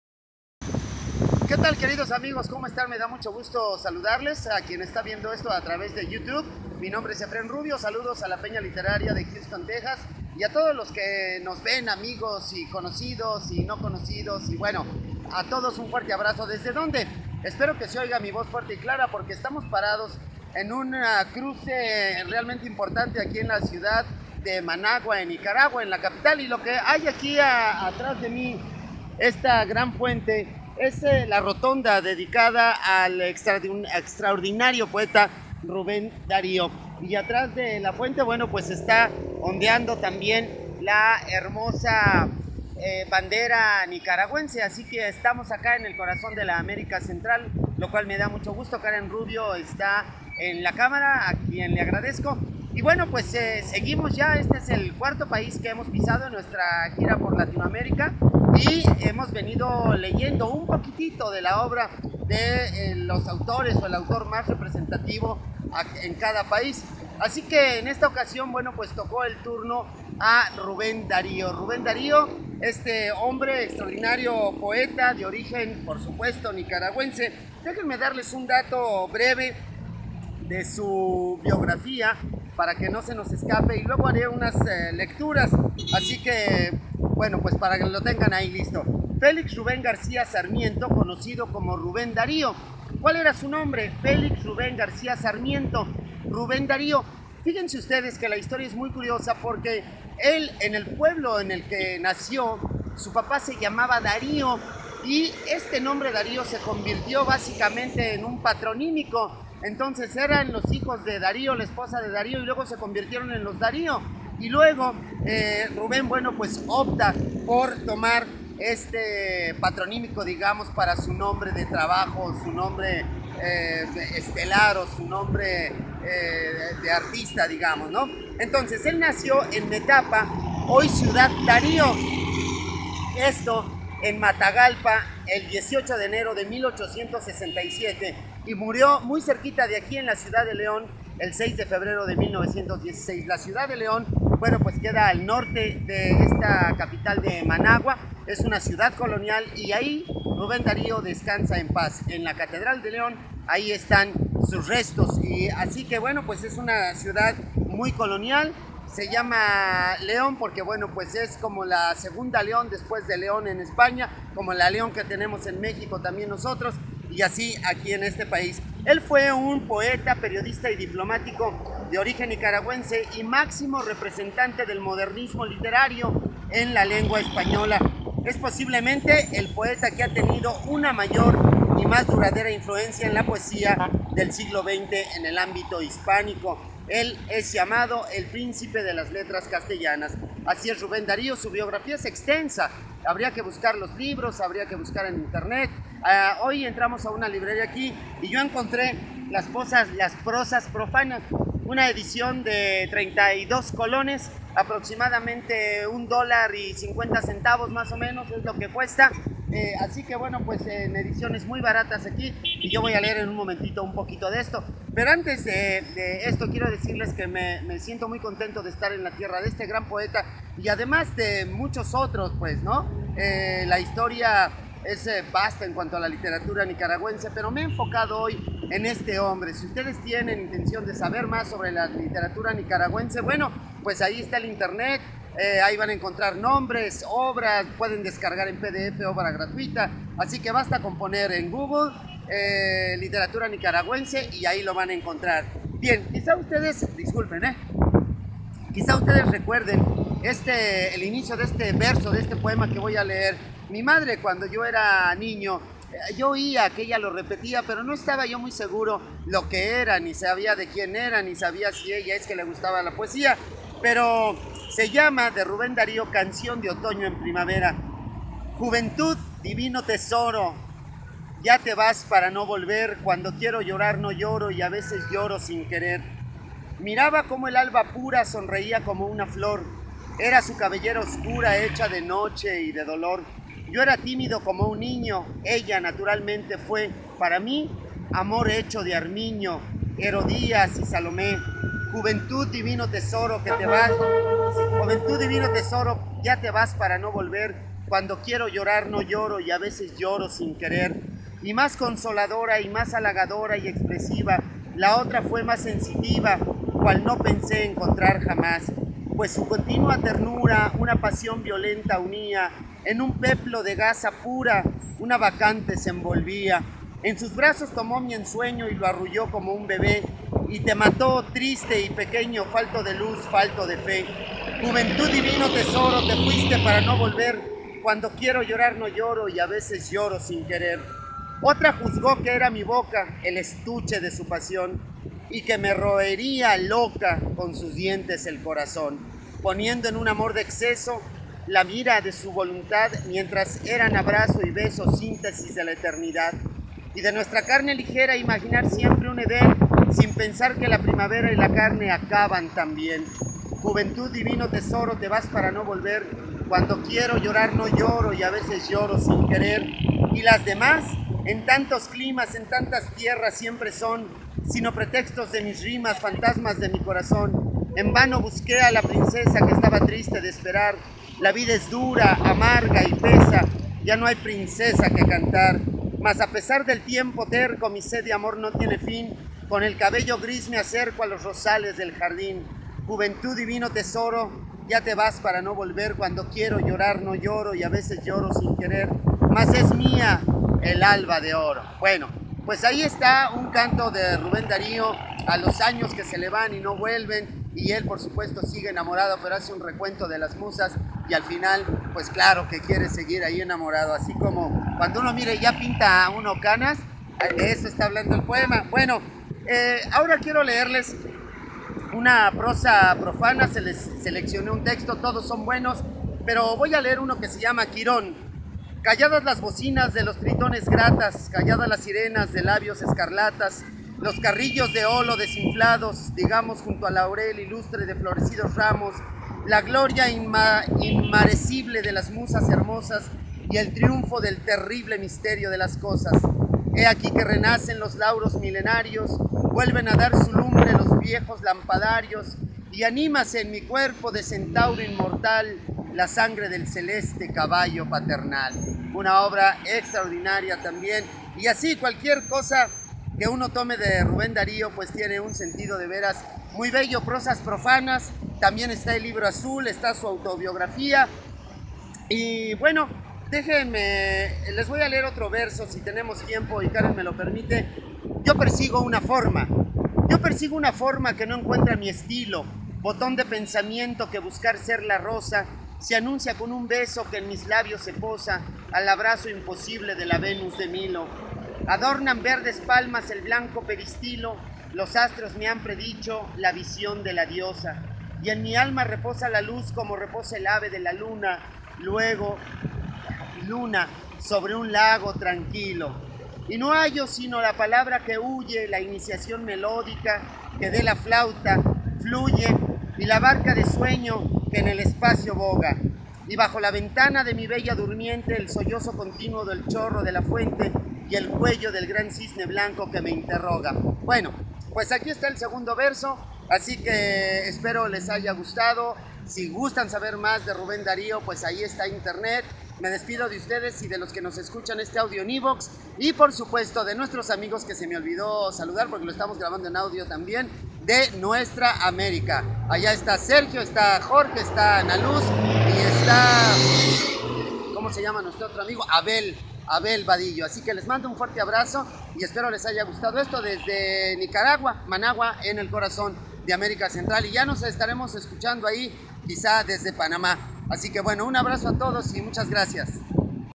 Literatura Nicaraguense Ruben Dario
Efren Rubio lee a Rubén Darío desde Managua Nicaragua en un recorrido por Latinoamérica.